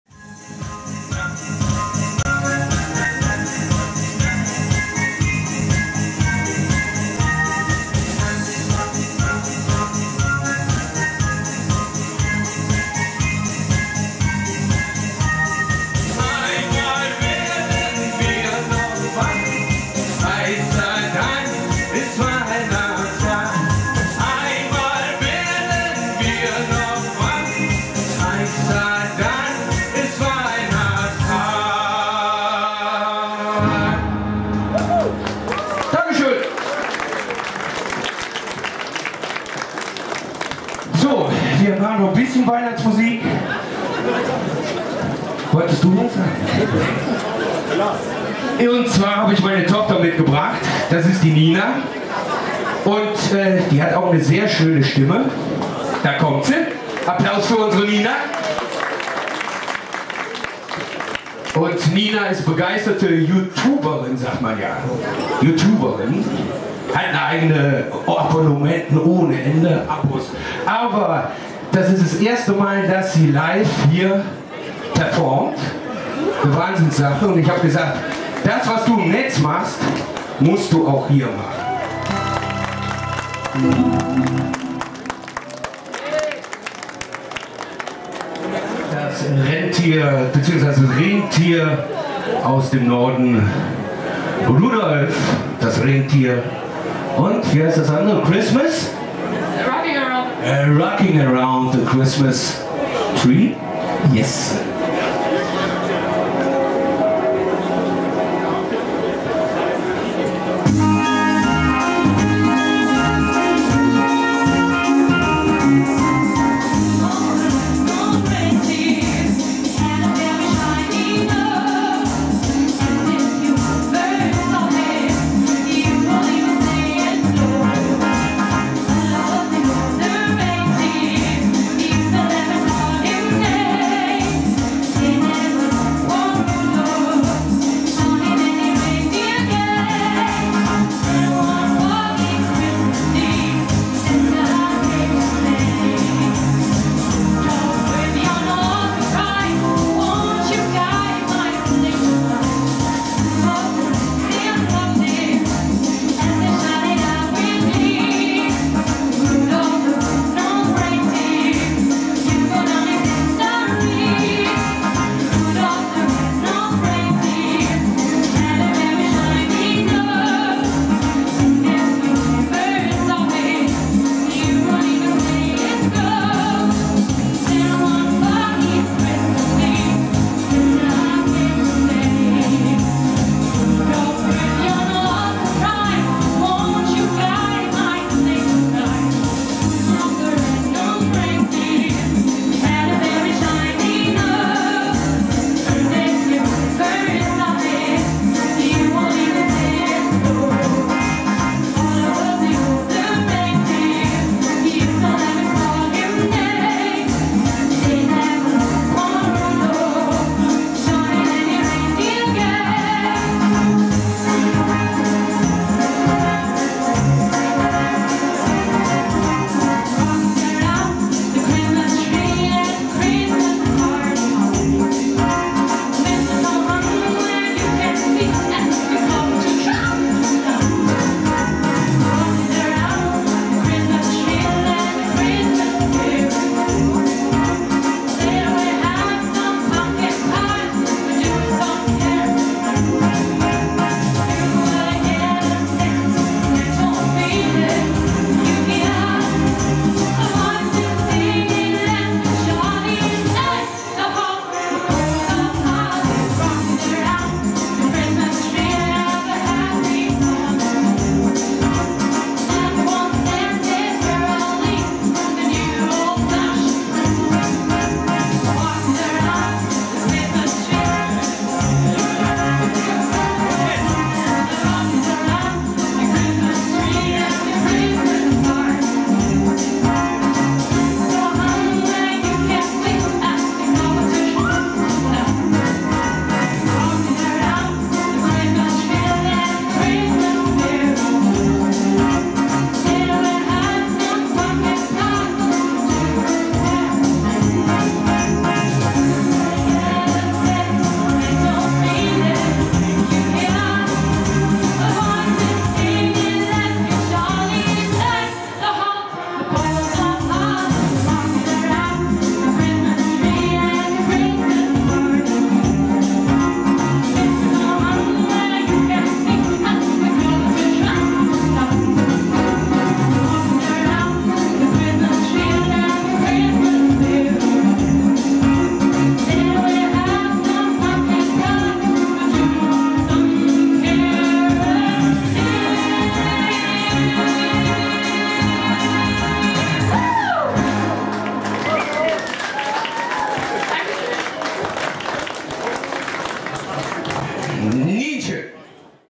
Weihnachtsmarkt Dortmund am 26.12.2015
Erster Auftritt mit meiner Tochter Nina.